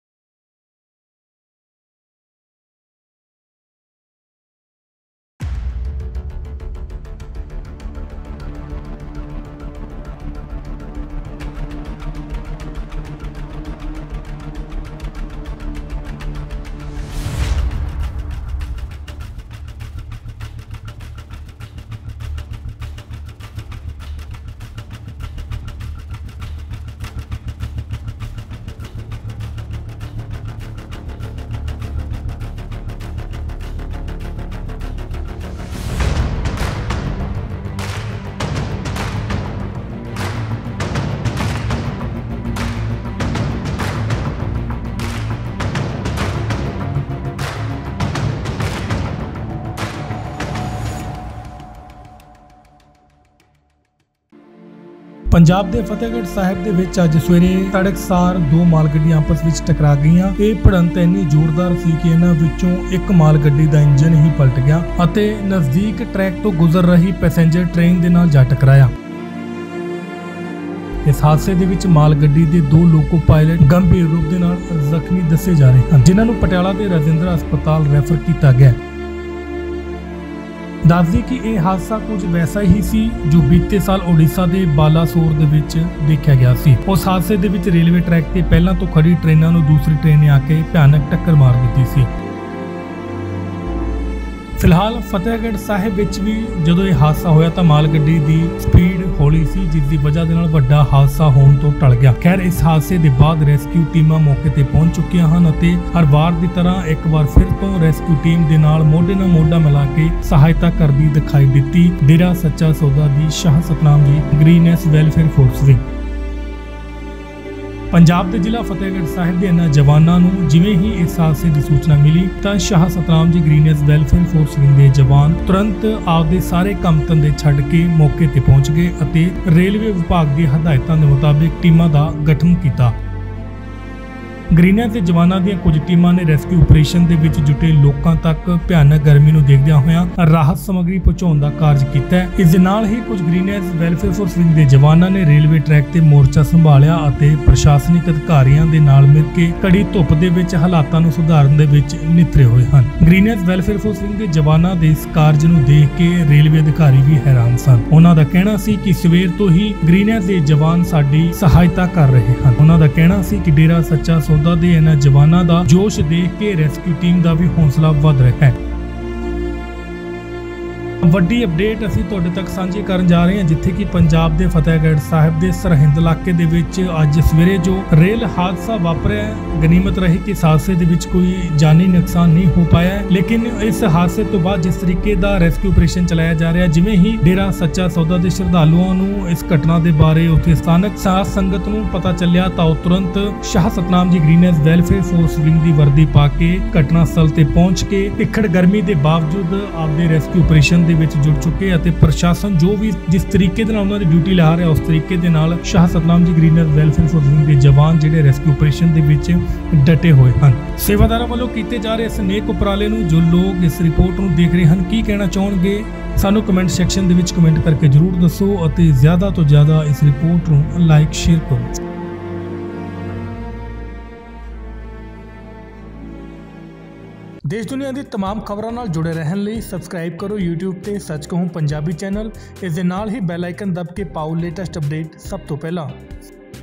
Punjab Rail Accident:ਰੇਲ ਹਾਦਸੇ ਦੀਆਂ ਤਾਜ਼ਾ ਤਸਵੀਰਾਂ | Dera ਸ਼ਰਧਾਲੂਆਂ ਨੇ ਸੰਭਾਲਿਆ ਮੋਰਚਾ | Green-S-Force
Punjab Rail Accident:ਰੇਲ ਹਾਦਸੇ ਦੀਆਂ ਤਾਜ਼ਾ ਤਸਵੀਰਾਂ | ਡੇਰਾ ਸ਼ਰਧਾਲੂਆਂ ਨੇ ਸੰਭਾਲਿਆ ਮੋਰਚਾ || Shah Satnam Ji Green S Welfare Force Wing || DSS Volunteers

Two goods trains collided with each other at around 4 am today in Fatehgarh Sahib, Punjab. The collision was so strong that the engine of one of the goods trains overturned and collided with a passenger train passing on the adjacent track. In this accident, two loco pilots of the goods train are said to be seriously injured. They were referred to Rajindra Hospital, Patiala. Let us tell you that this accident was similar to the one that happened in Balasore, Odisha last year. In that accident, another train hit a train already standing on the railway track. During this collision, a third train was passing from there, it also became a victim of this accident. In which about 300 passengers were killed. But when this accident happened in Fatehgarh Sahib, the speed of the goods train was slow, due to which a major accident was averted. Well, after this accident, rescue teams have reached the spot and like every time, once again the soldiers of Shah Satnam Ji Green S Welfare Force Wing of Dera Sacha Sauda were seen helping shoulder to shoulder with the rescue team. As soon as these soldiers of District Fatehgarh Sahib, Punjab received information about this accident, these Green S soldiers immediately reached the spot and formed teams as per the instructions of the Railway Department.